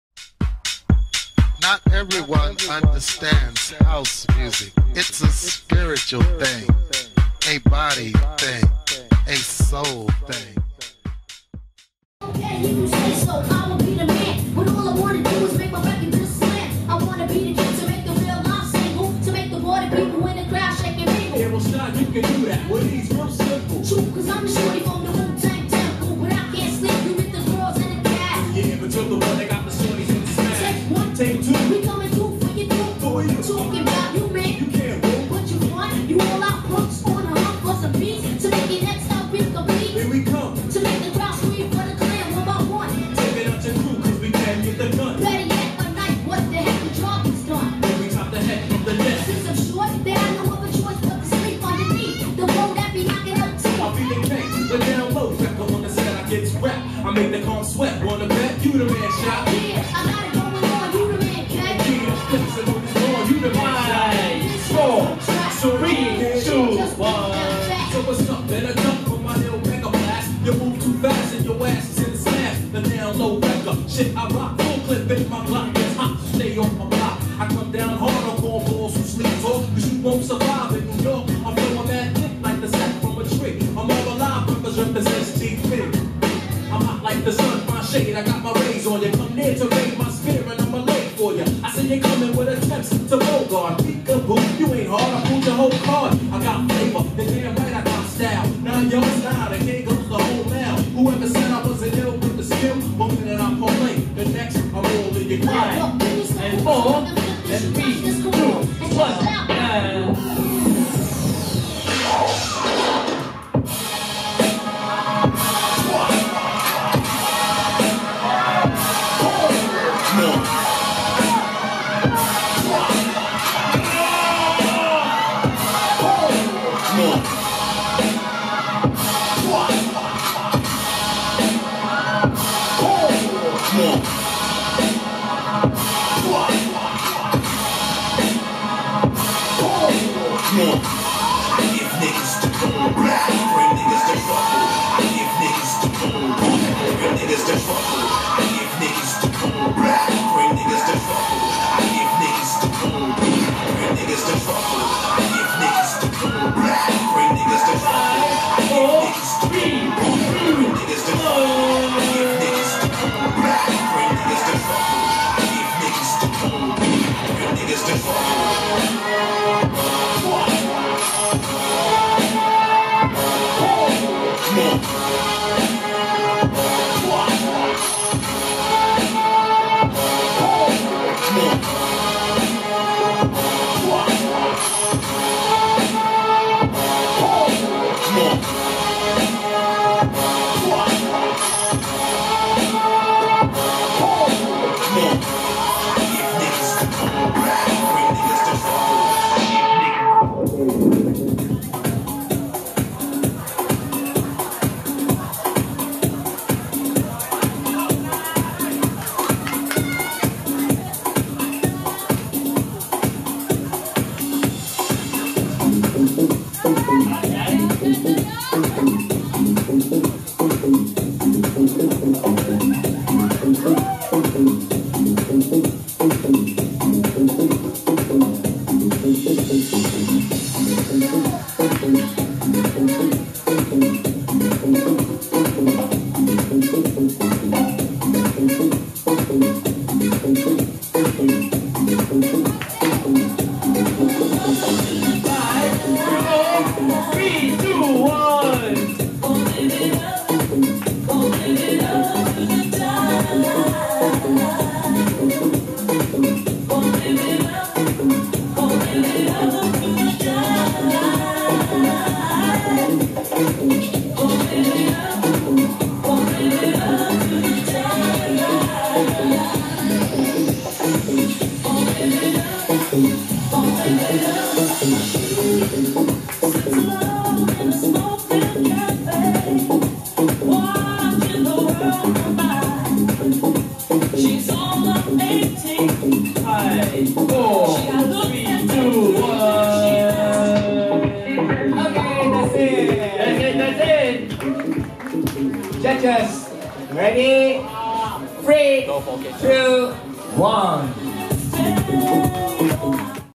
Soul Session | Allstyle 3on3 BEST16 - Happy Mothers Day vs 今晚睇女神
Soul Session : House 1on1 / All style 3on3 Battle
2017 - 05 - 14

DJ : Shan.S (USA) / Monsta Kei (HongKong)
MC : Deegor / Alton

House 1 VS 1
Judge : Alesya (Russia) , Santa(Japan)
Battle Guest : Miyu & Jumpei @Alaventa (Japan)

Allstyle 3 VS 3
Judge : Alesya (Russia) / 小四 (Taiwan) / Yellow (HongKong)
Battle Guest : 泰仔 Zero Ivan (Macau)